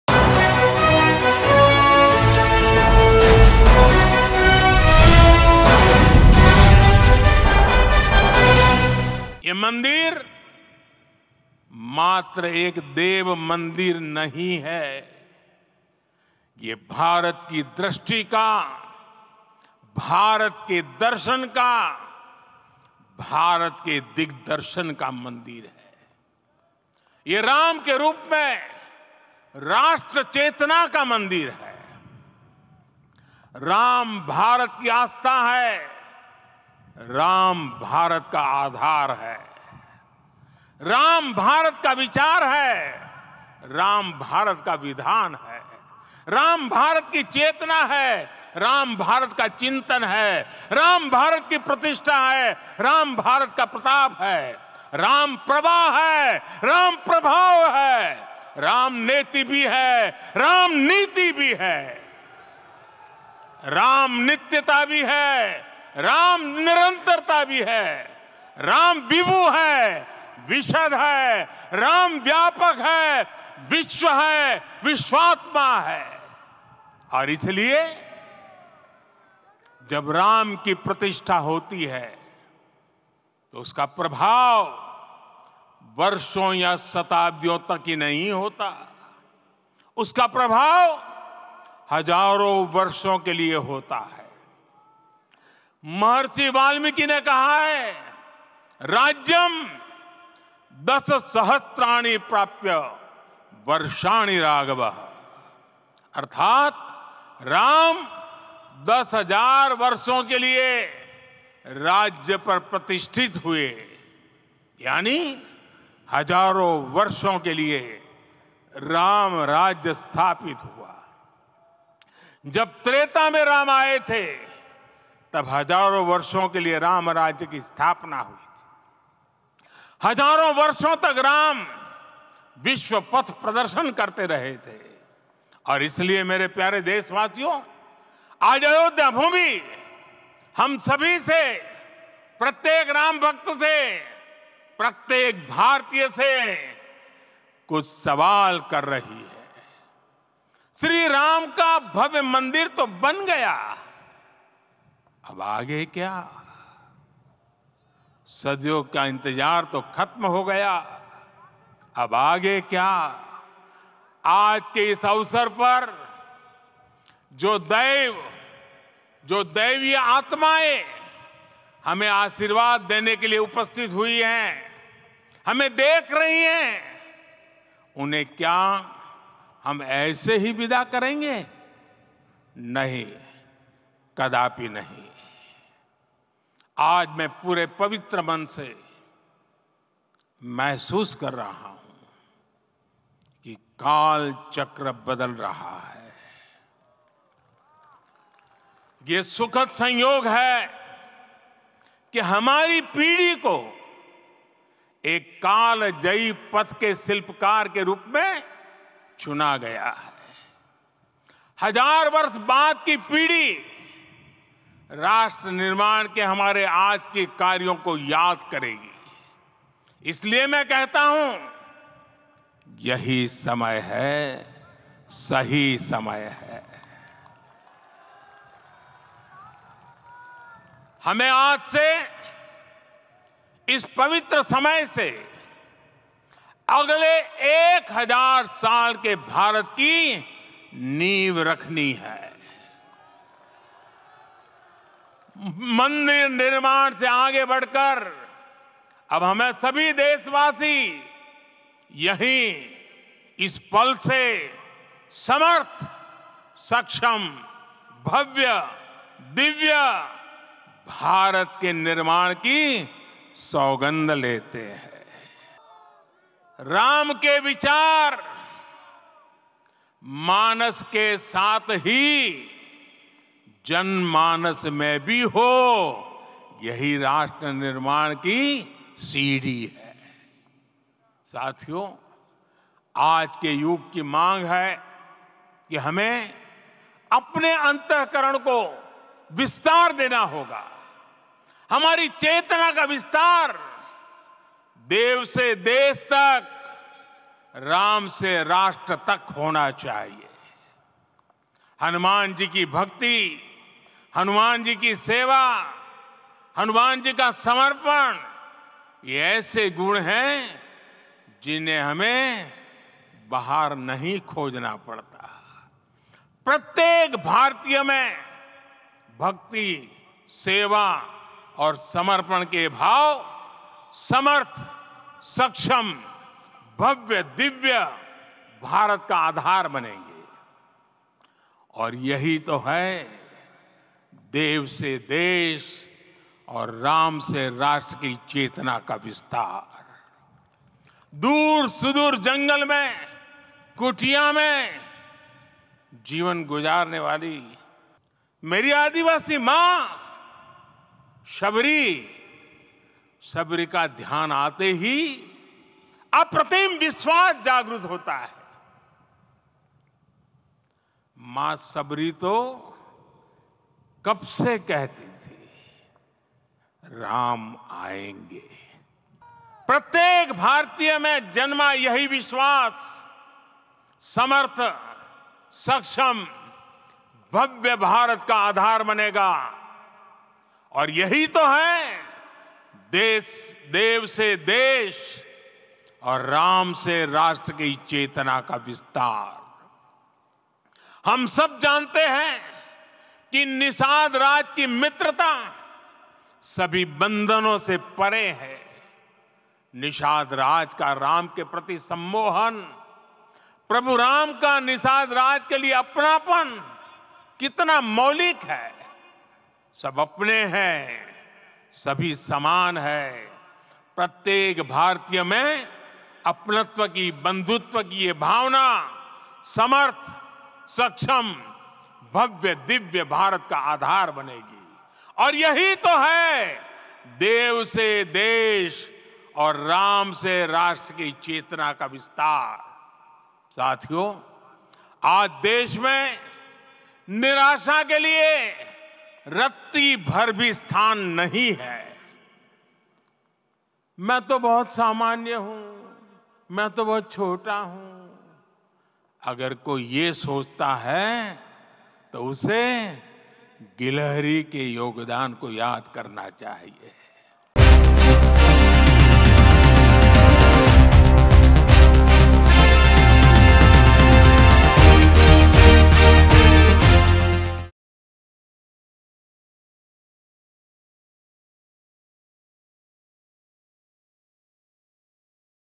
PM Modi Speech At Ayodhya Ram Mandir Inauguration Ceremony | Ram Temple | Ram Lalla | Mango News
PM Modi Speech At Ayodhya Ram Mandir Inauguration Ceremony.